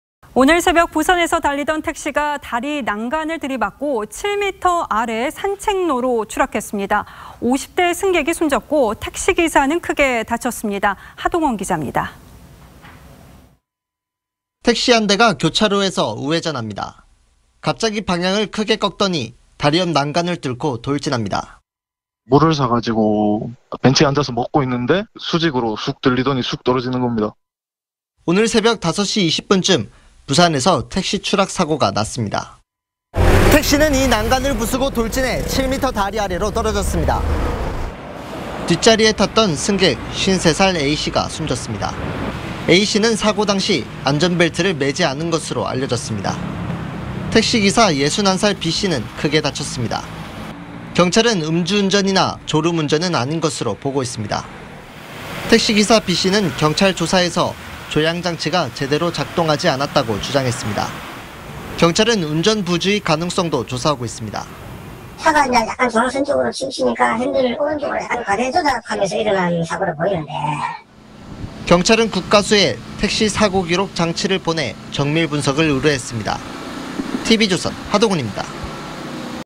다리 난간 뚫고 택시 7m 추락, 출근길 승객 '참변'
오늘 새벽 부산에서 달리던 택시가 다리 난간을 들이받고 7m 아래 산책로로 추락했습니다. 50대 승객이 숨졌고, 택시기사는 크게 다쳤습니다.

하동원 기자입니다.